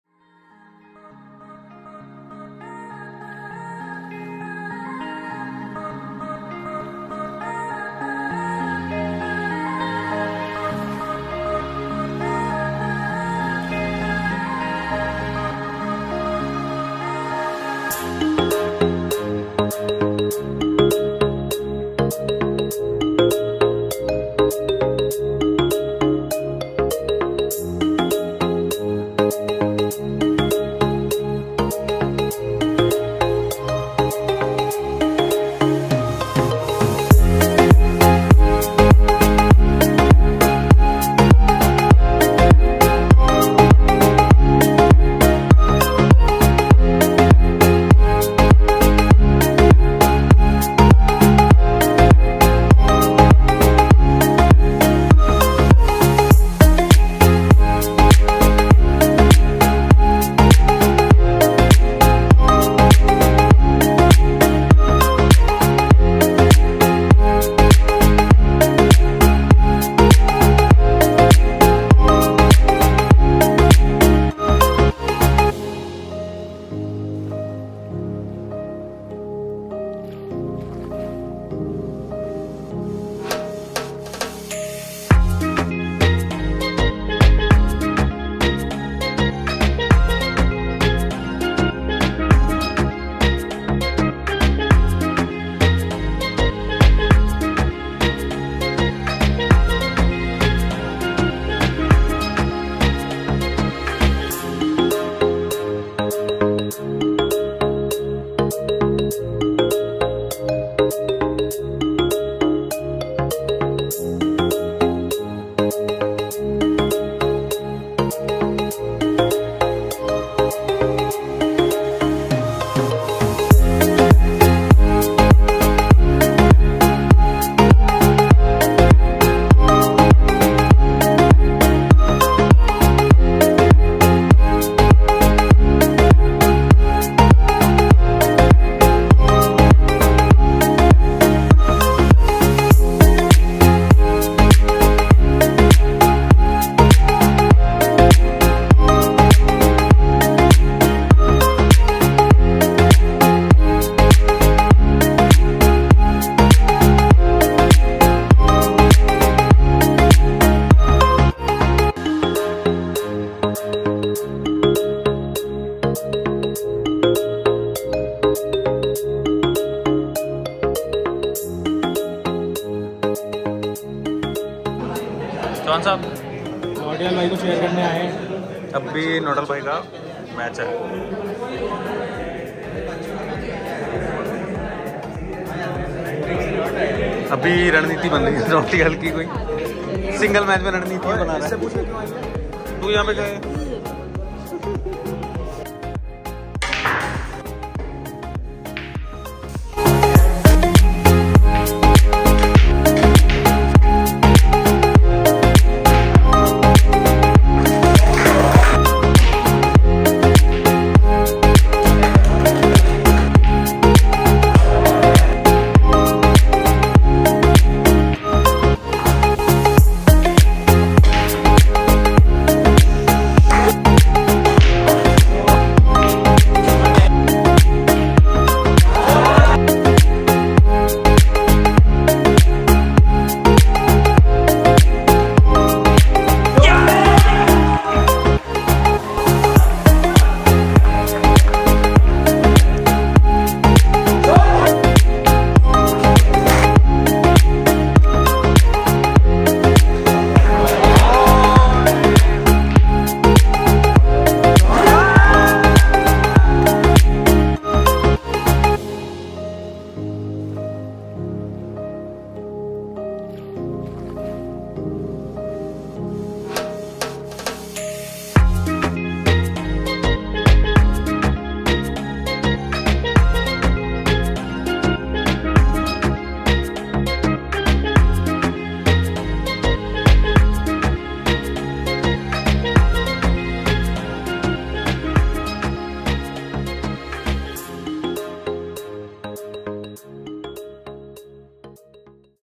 FESTIVAL SEASON PART 2 | IIM AMRITSAR | VLOG 12
As the festival season kicks off, a fresh start brings about new adventures, a different atmosphere, and exciting fun.